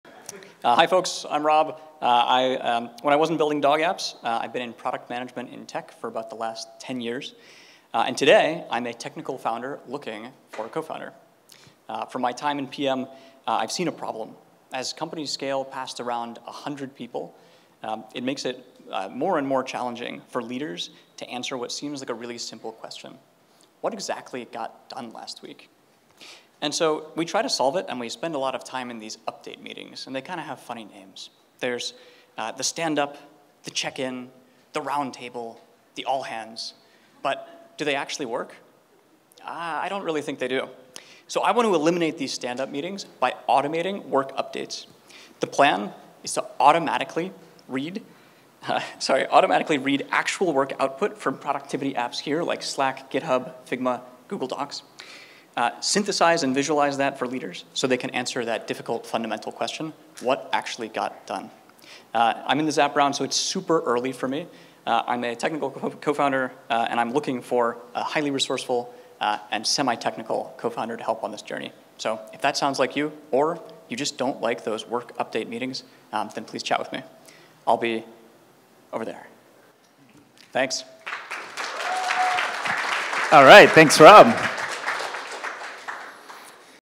Rob Balian at Founders You Should Know
Rob's looking for a co-founder!
Founder: Rob Balian
What it is: Eliminates meetings through automatic work updates